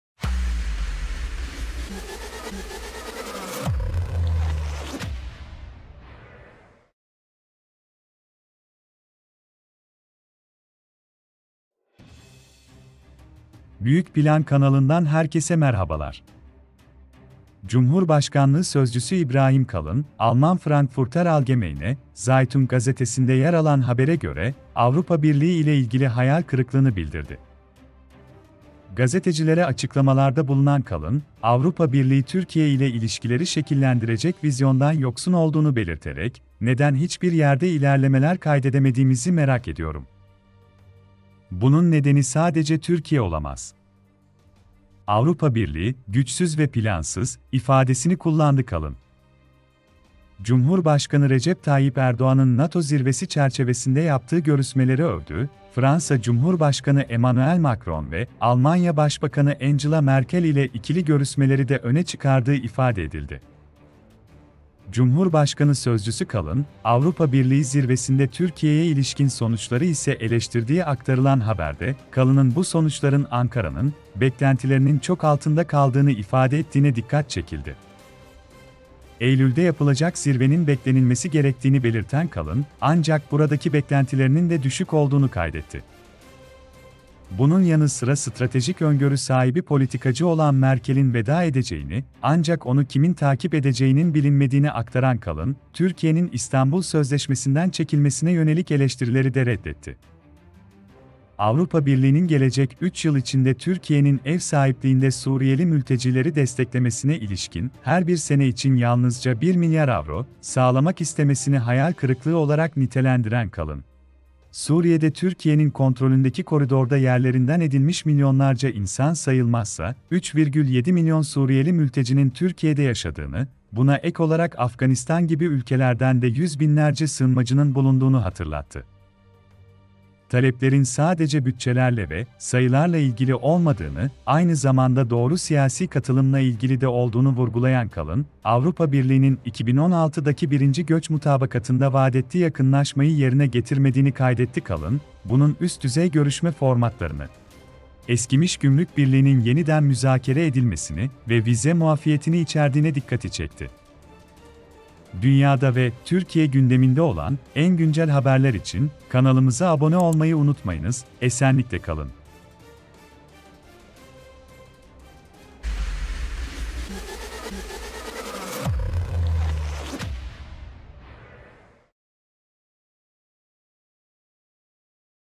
ALMAN BASININA KONUŞAN İBRAHİM KALIN...TOKAT GİBİ SÖZLER HİÇ ACIMADI...
AB'nin güçsüz ve plansız olduğunu vurgulayan Cumhurbaşkanlığı Sözcüsü İbrahim Kalın, "Neden hiçbir yerde ilerlemeler kaydedemediğimizi merak ediyorum.

türkiye siyaset tarihi,
türkiye siyaset,